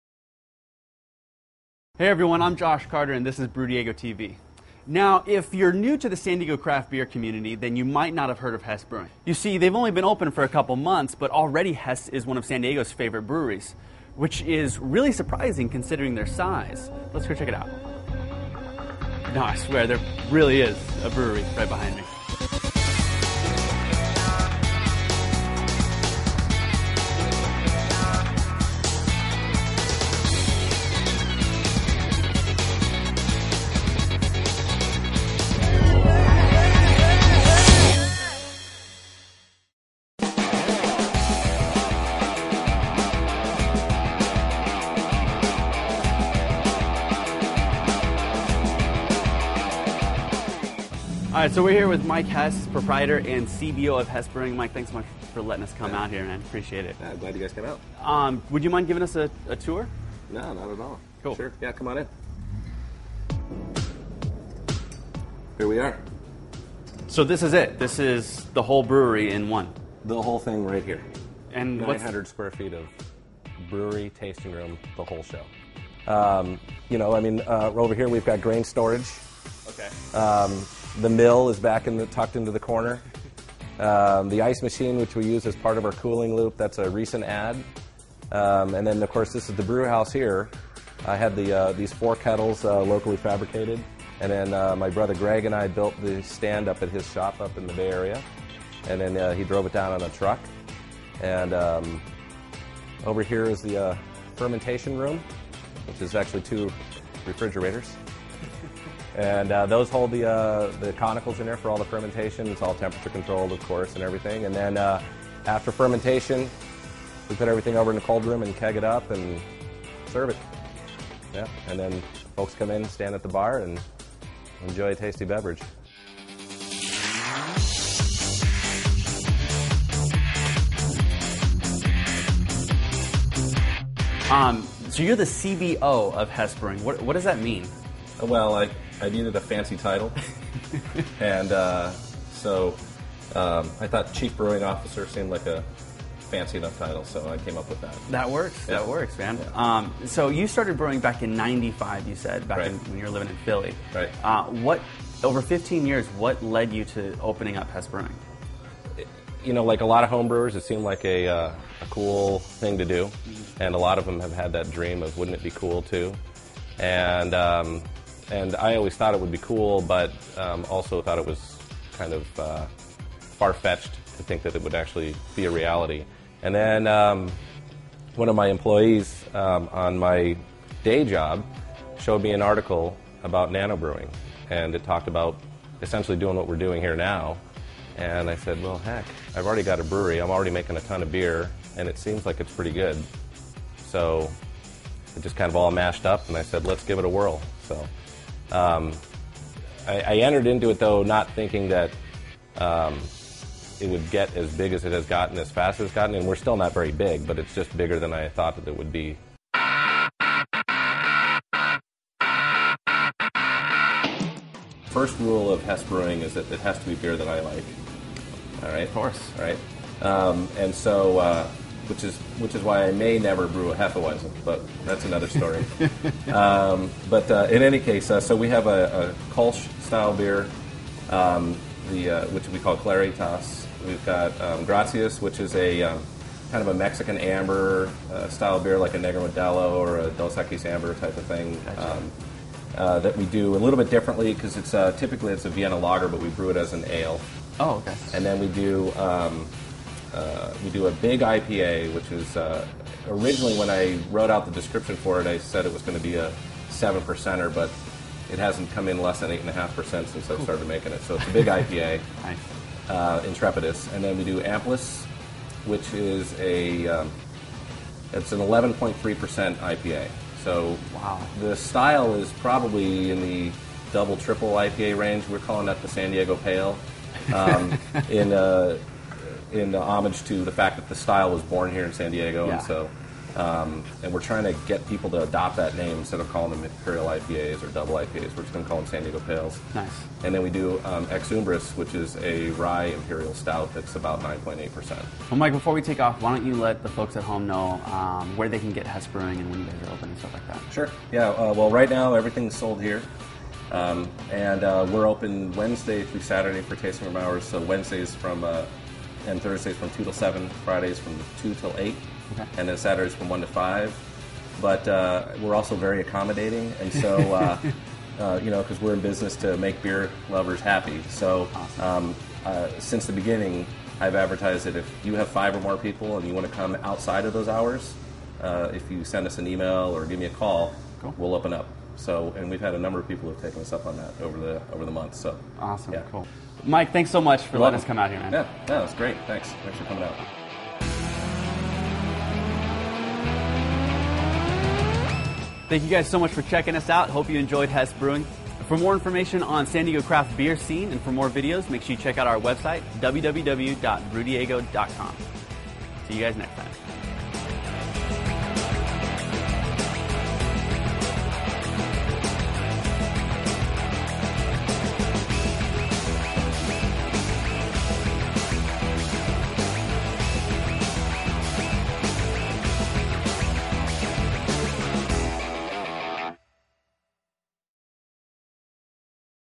BrewDiego | A Tour of Hess Brewing
BrewDiego's Josh Carter interviews Mike Hess, the CBO of Hess Brewing.

For the beer geeks: Check out the Hess Brewing profile page on BrewDiego for more information on Hess Brewing

For the video geeks:  This was shoot using a T2i, 5Dmii, H4N Zoom, and a variety of lenses.